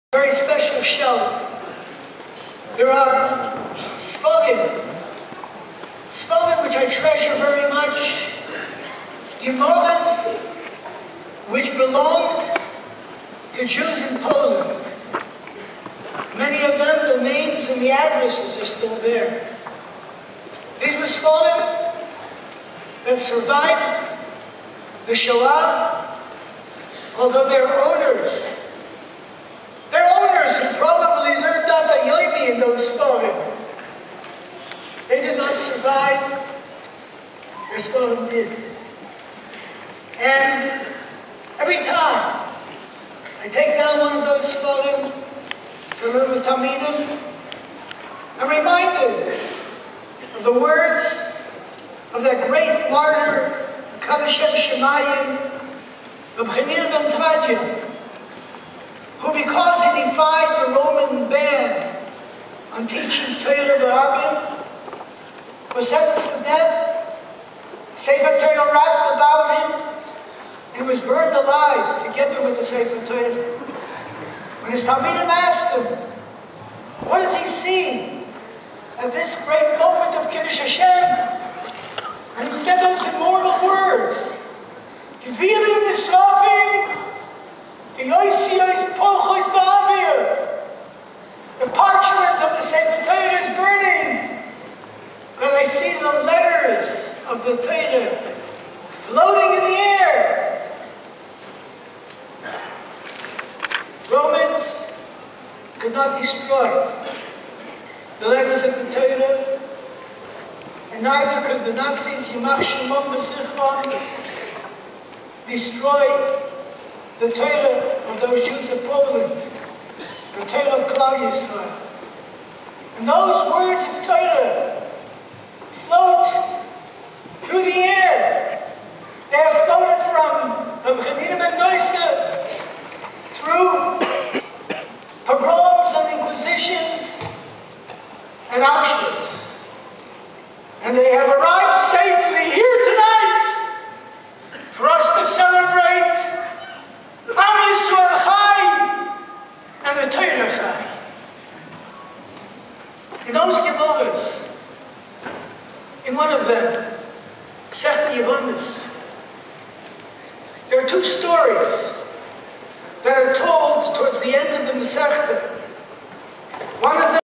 12TH DAFYOMI SIYUM HASHAS "Ohr Somayach" BD'H Remembers HaRav Mendel Weinbach z'tl, Jerusalem!
BD'H Ohr Somayach Rav Mendel Weinbach Tz'l leaves us in mourning! He was my Rosh Hayeshiva and constantly influenced my life with many hashgacha pratis meetings like he spoke at the Siyum Hashas and chinuch shmoozes we went too , may he look out for our generation !